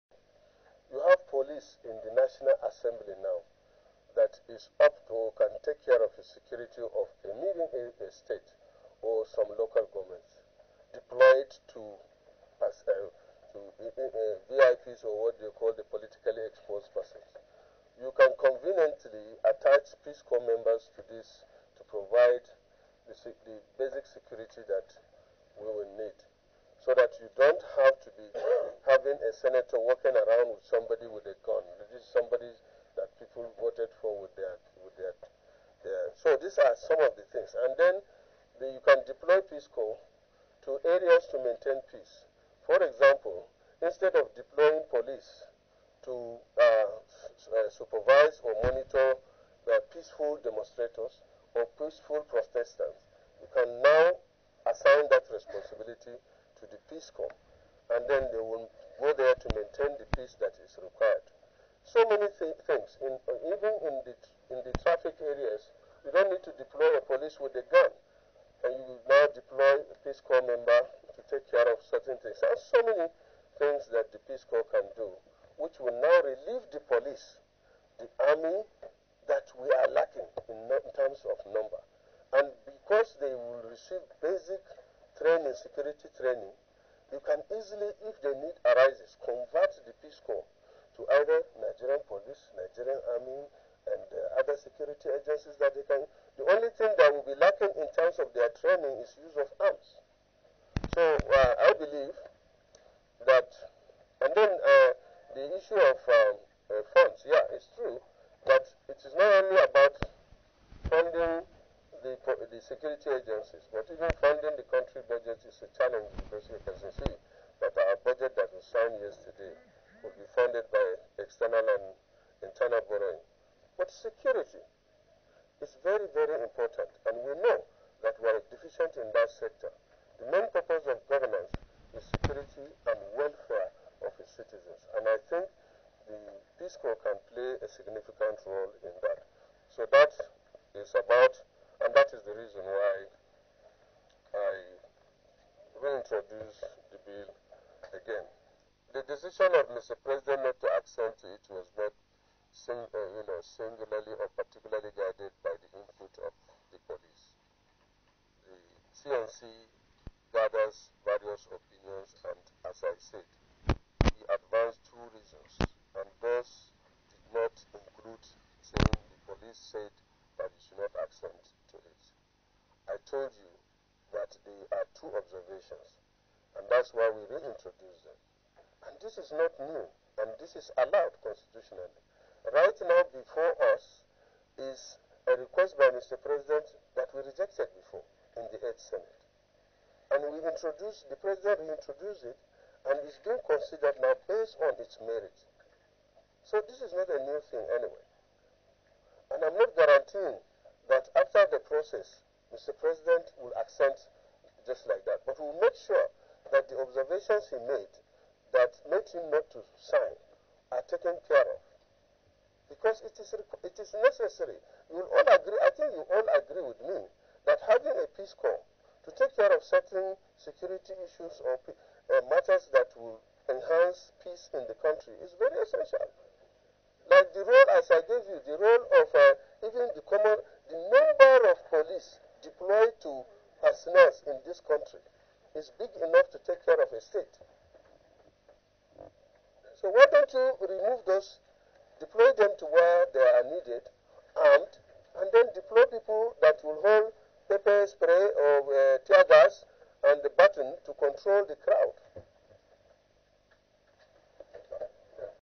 Just In : President Buhari May Sign Peace Corps Bill, If Passed Again
Chairman, Senate Committee on Army, Senator Aliyu Ndume, has advised that the presence of Peace Corps officers around the lawmakers in the National Assembly would be more civil than the regular armed Police.

Ndume posited that the arrays of Police personnel attached to Very Important Persons (VIPs) and lawmakers in the National Assembly alone was enough to man the security of a state.

The lawmaker stated this on Wednesday, while briefing pressmen at the Senate Press Centre on the reason why he reintroduced the Bill for an Act to establish Nigerian Peace Corps.